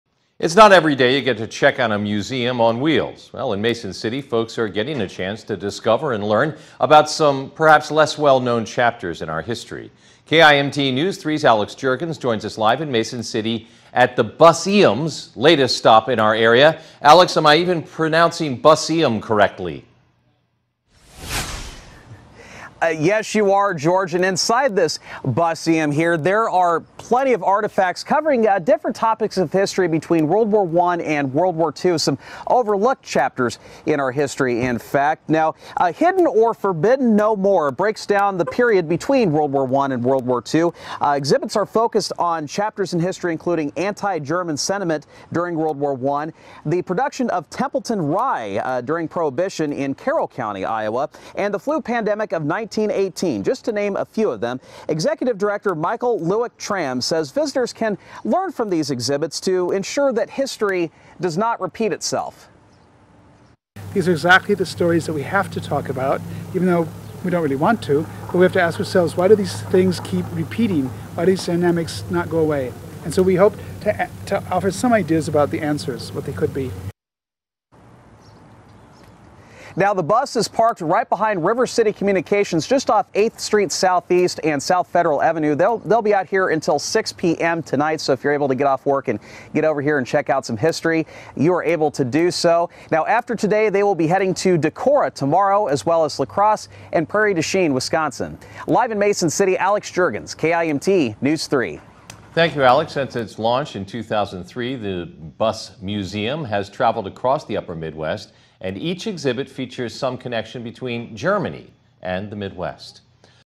Bus-eum Stops By Mason City, Iowa | KIMT3
TV feature KIMT3 Jul 6, 2021
In this extract from a report presented by Alex Jirgens, you get to know more about the history mobile, known as the Bus-Eum.